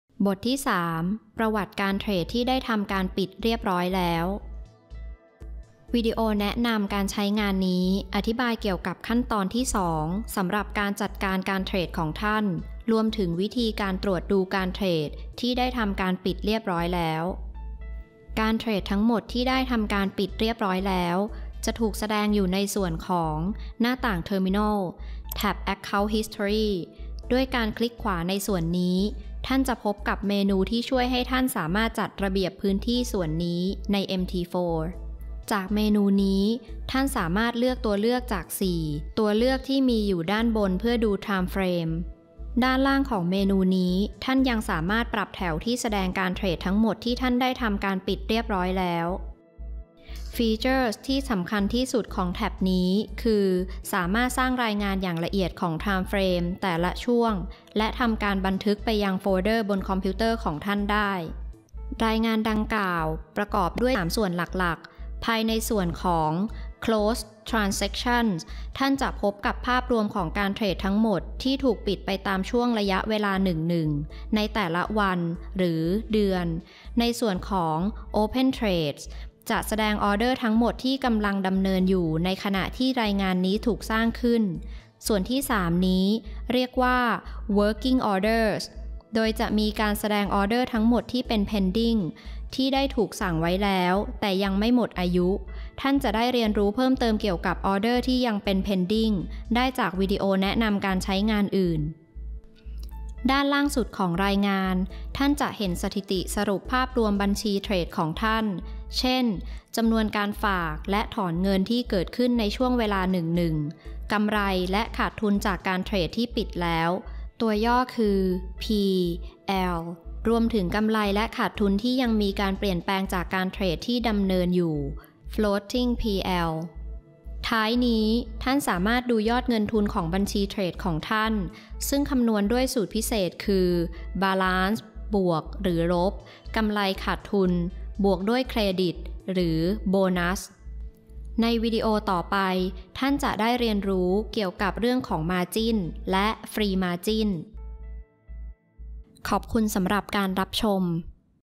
XM.COM - แนะนำการใช้งาน MT4 - ประวัติการเทรดที่ได้ทำการปิดเรียบร้อยแล้ว
คำเตือนเกี่ยวกับความเสี่ยง
เนื้อหาจะขึ้นอยู่กับประเทศที่พำนักของคุณและผู้ที่มีหน้าที่ในการกำกับดูแลบริการทางการเงิน

วิดีโอแนะนำการใช้งานทีละขั้นตอนเกี่ยวกับวิธีการใช้งานประวัติของบัญชีใน MT4 และวิธีการดูการเทรดที่ปิดไปแล้วใน MT4